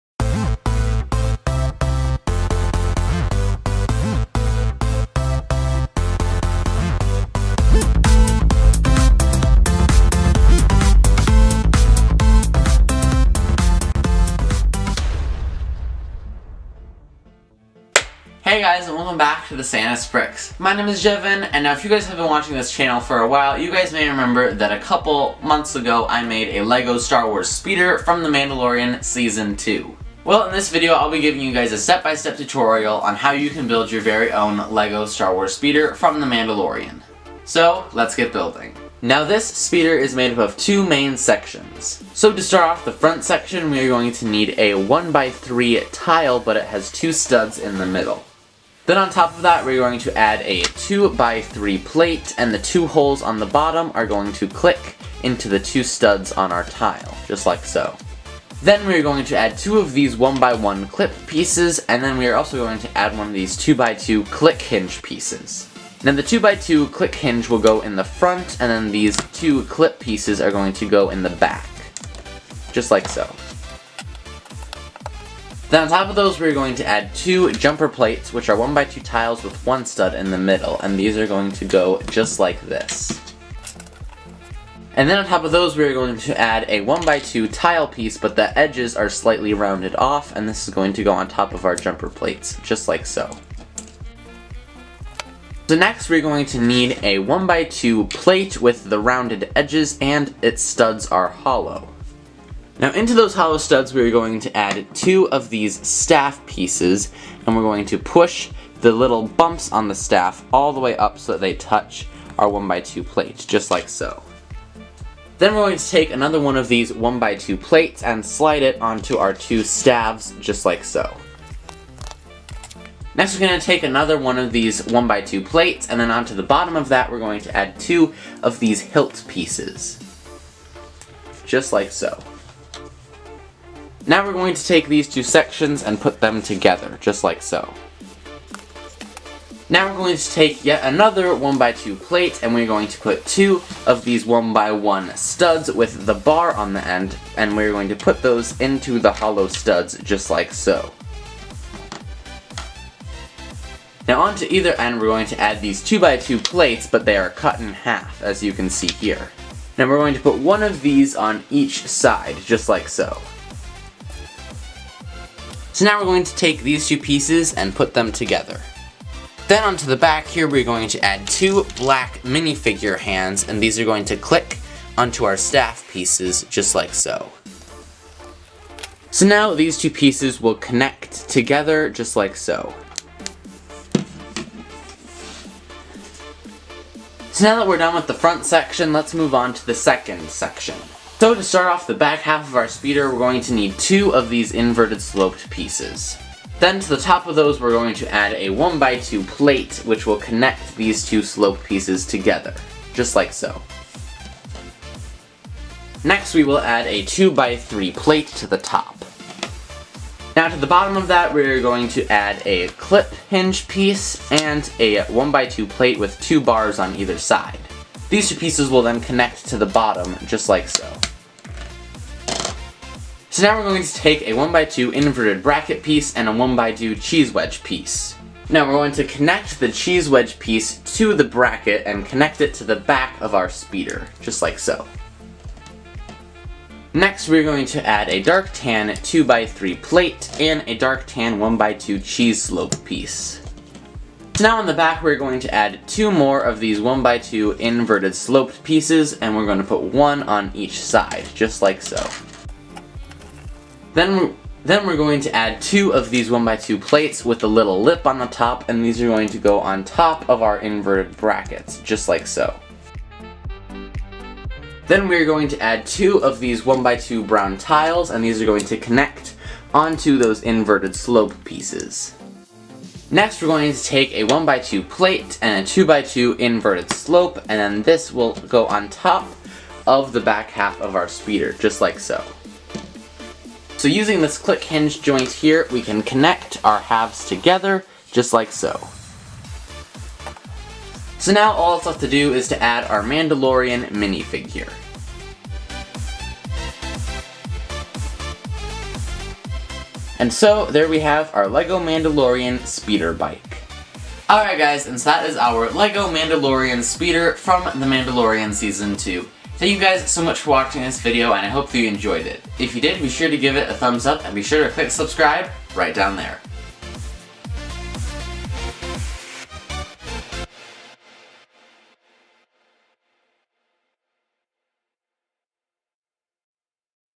LEGO Mandalorian Speeder Bike
In this video, I'll show you how to build you own speeder bike from the Mandalorian Season 2.